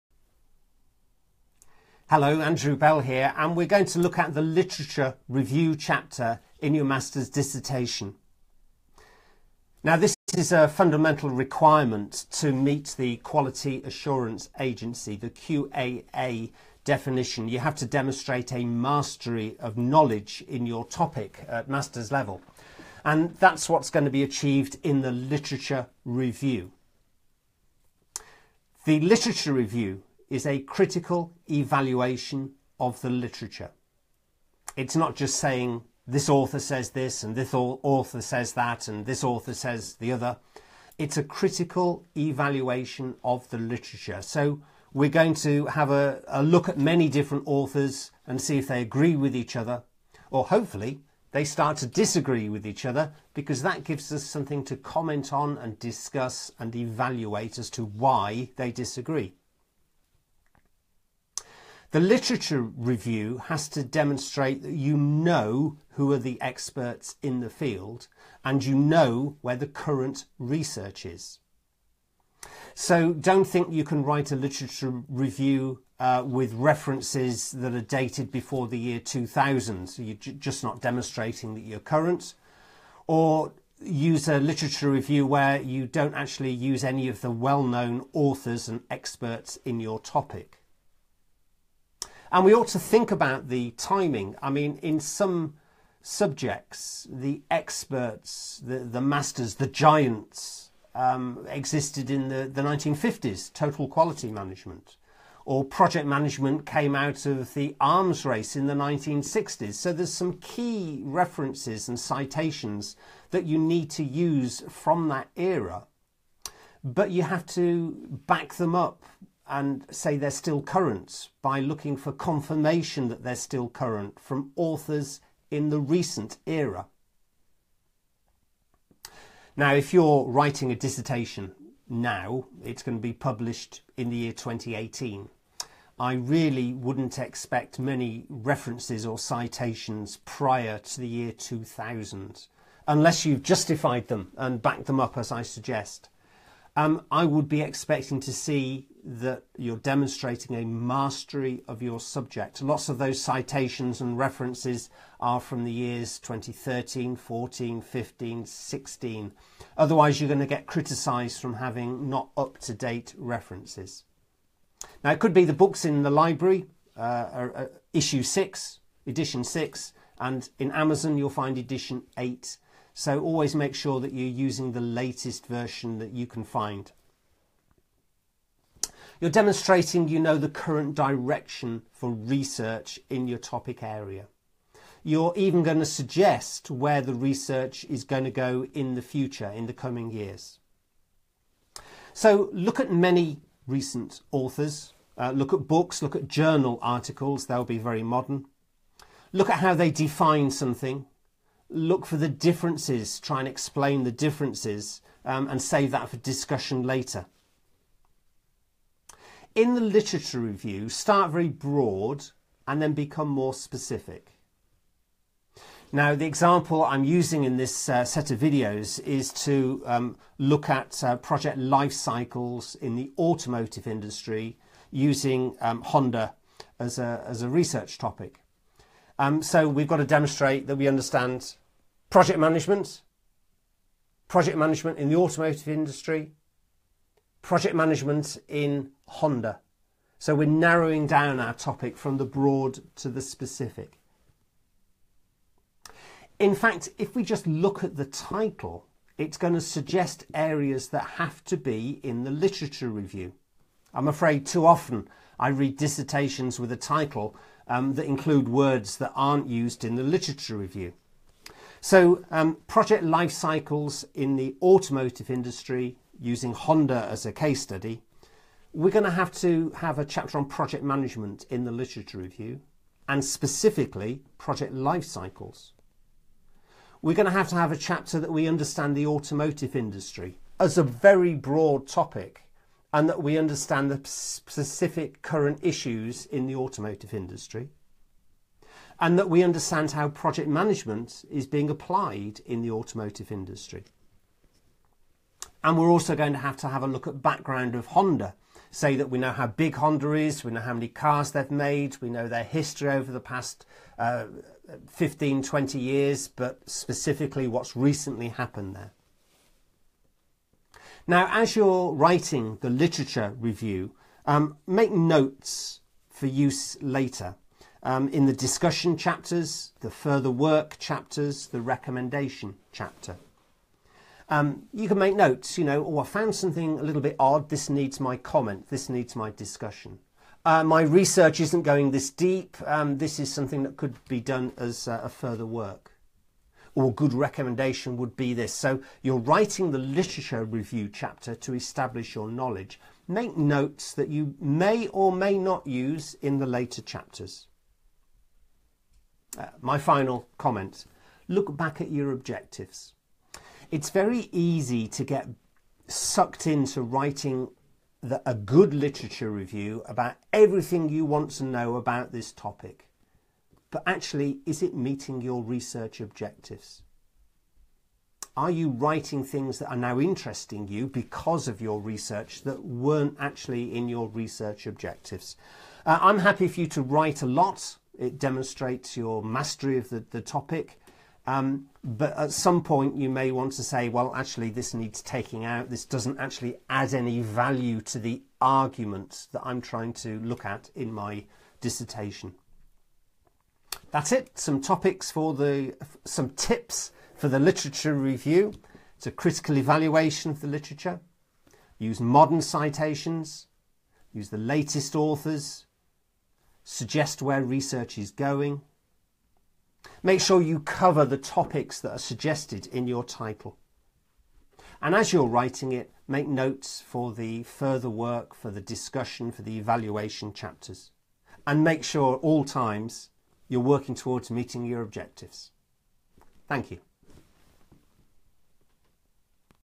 Literature Review in a Masters Dissertation
Literature Review in a Masters Dissertation. A look at tips for witing the Literature Review Chapter. What is expected, modern citations and references, focusing the research, meeting objectives.